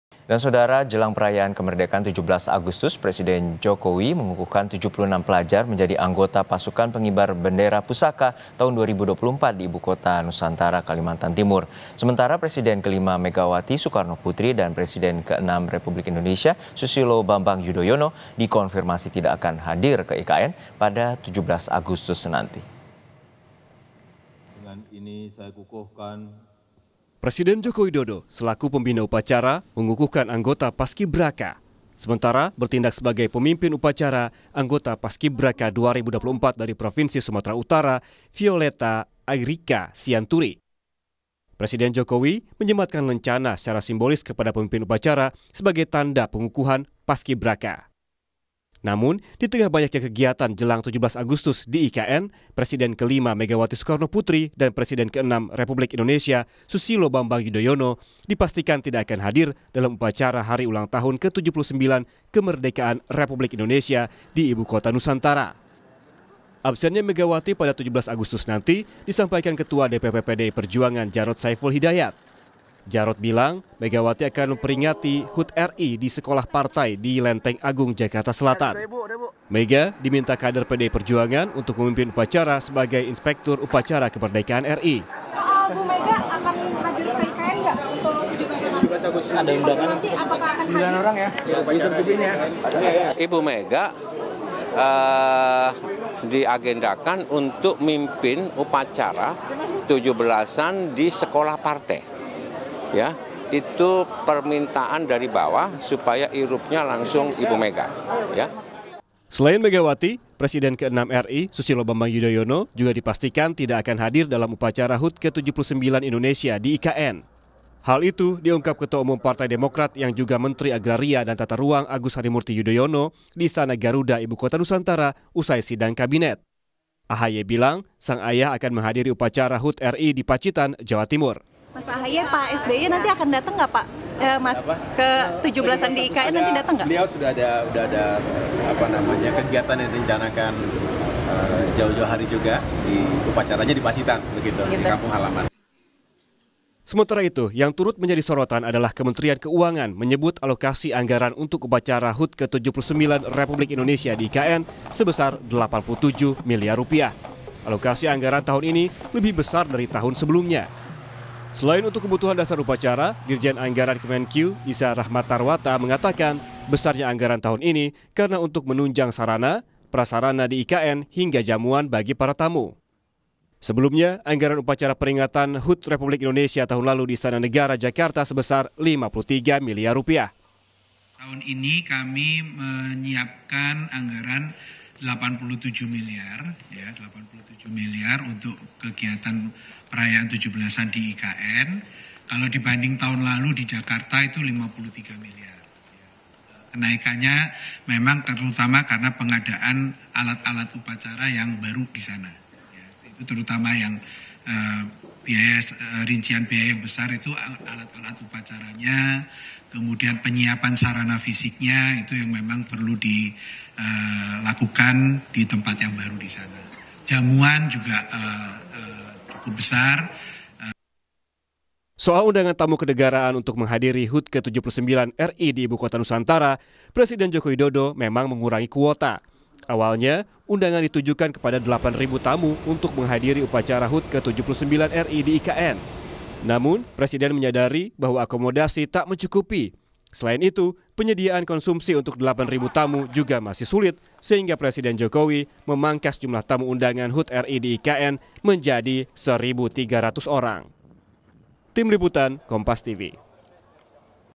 Megawati dan SBY Dikonfirmasi Bakal Absen Dalam Upacara HUT RI di IKN, Ini Alasannya!
KOMPAS. TV - Menjelang perayaan Kemerdekaan 17 Agustus, Presiden Joko Widodo mengukuhkan 76 pelajar sebagai anggota Pasukan Pengibar Bendera Pusaka (Paskibraka) 2024 di Ibu Kota Nusantara, Kalimantan Timur. Sebagai Pembina Upacara, Presiden Jokowi menyematkan lencana simbolis kepada pemimpin upacara, Violetha Agryka Sianturi dari Sumatera Utara.

Namun, Presiden ke-5 RI Megawati Soekarnoputri dan Presiden ke-6 RI Susilo Bambang Yudhoyono dipastikan tidak akan hadir dalam upacara HUT ke-79 Kemerdekaan RI di Ibu Kota Nusantara. Ketua DPP PDIP, Djarot Saiful Hidayat, menyampaikan bahwa Megawati akan memimpin upacara di Sekolah Partai, Jakarta Selatan. Sementara itu, Agus Harimurti Yudhoyono mengungkapkan bahwa SBY akan memperingati HUT RI di Pacitan, Jawa Timur.